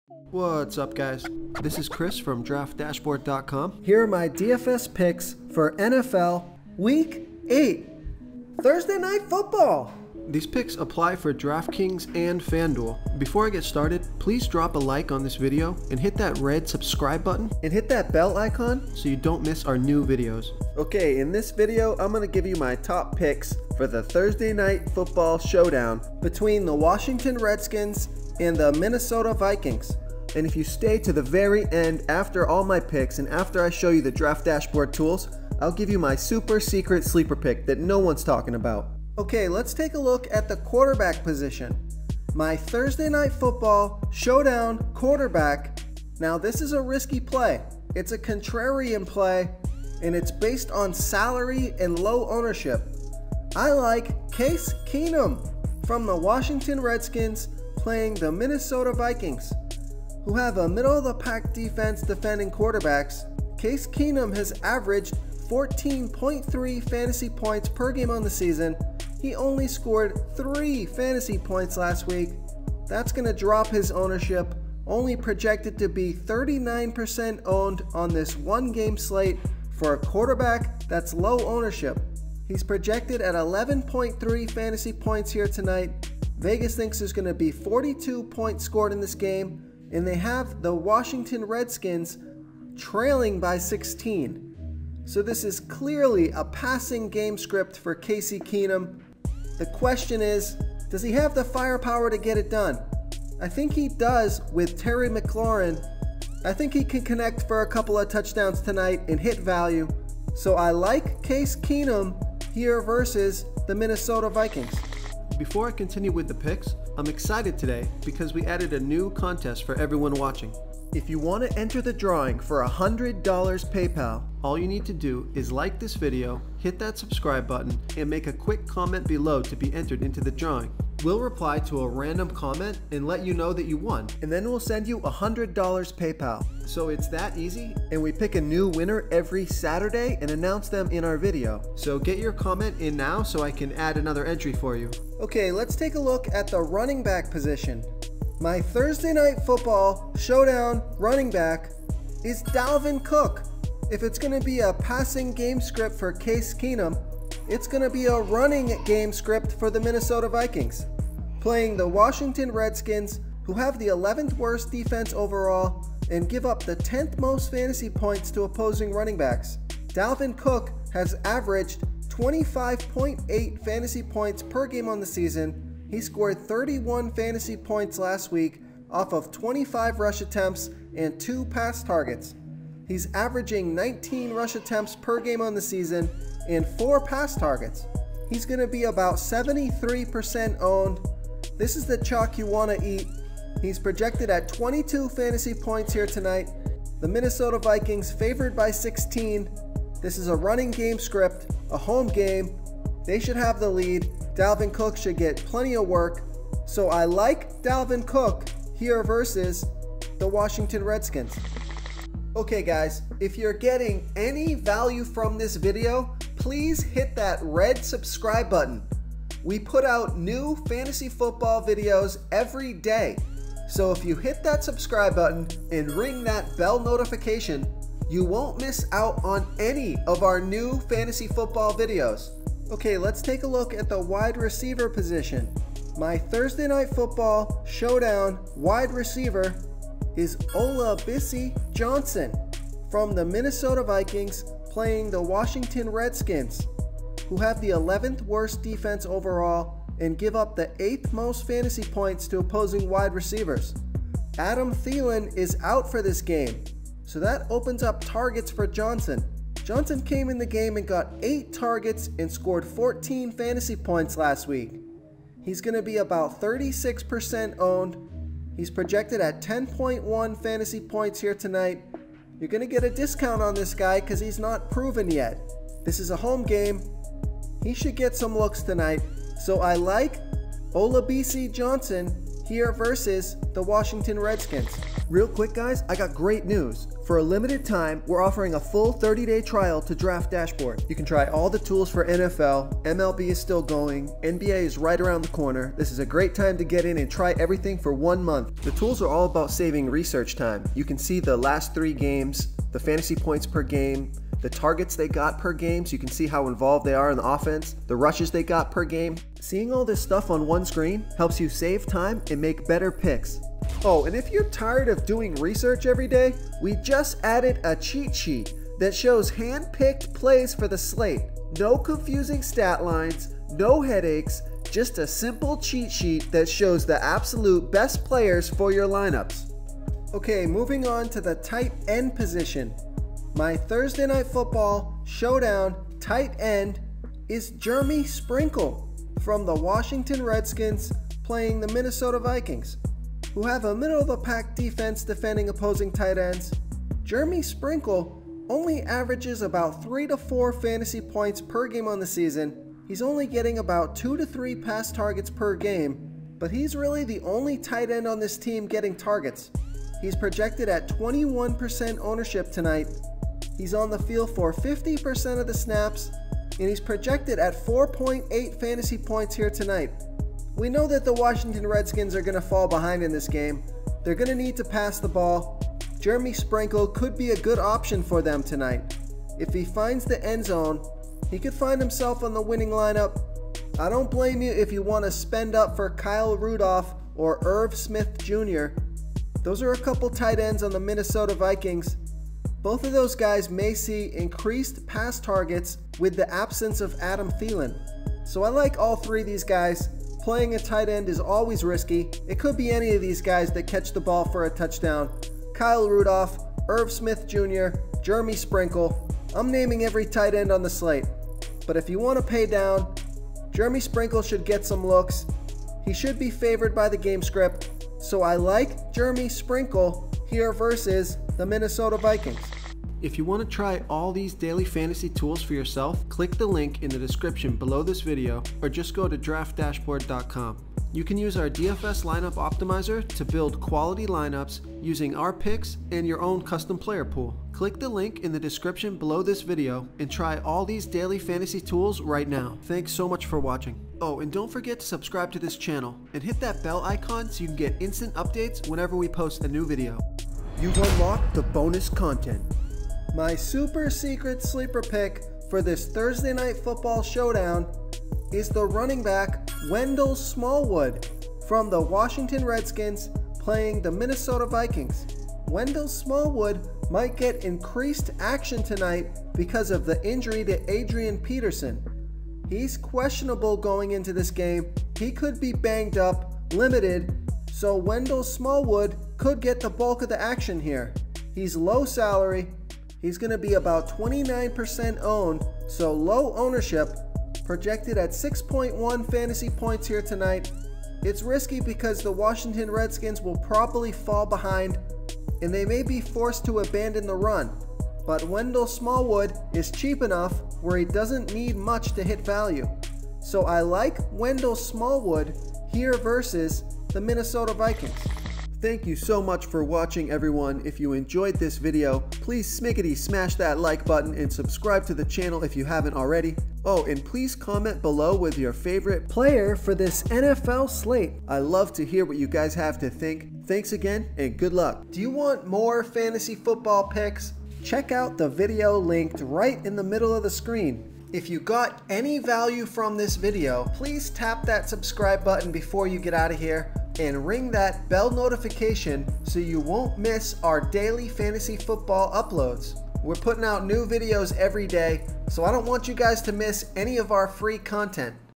DraftKings Picks NFL Week 8 Thursday Night Football TNF Showdown
DraftKings Picks NFL Week 8 Thursday Night Football TNF Showdown! Top NFL DFS Picks for Today Thursday October 24, 2019. Try our DFS Lineup Optimizer, Strategy, Advice and Daily Fantasy Football FanDuel Picks for DFS Lineups Tonight!